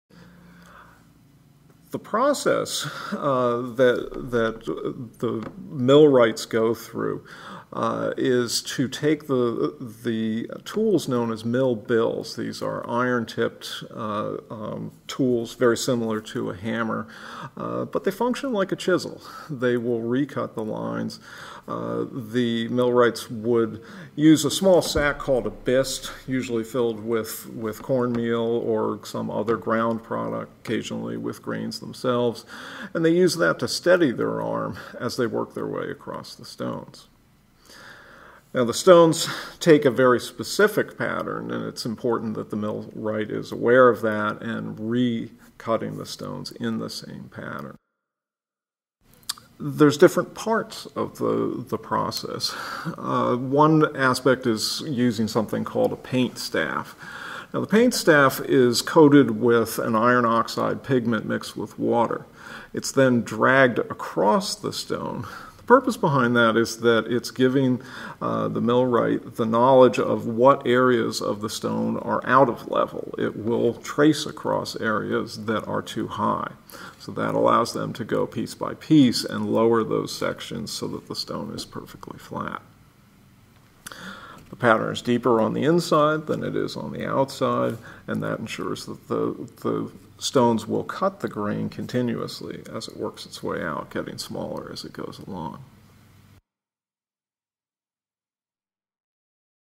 Philipsburg Manor's millstones: All dressed up to go round and round
Just one set of Philipsburg Manor's millstones weigh around 4,100 pounds, so they generally don't get out much. Nonetheless, the daily grind wears them down just the same, hence the need for dressing, and not in haute couture. Stone dressing refers to work millers carry out on the surface of millstones so they will more efficiently and cleanly do their job of cutting grain. Check out this video for a full explanation and demonstration of the process.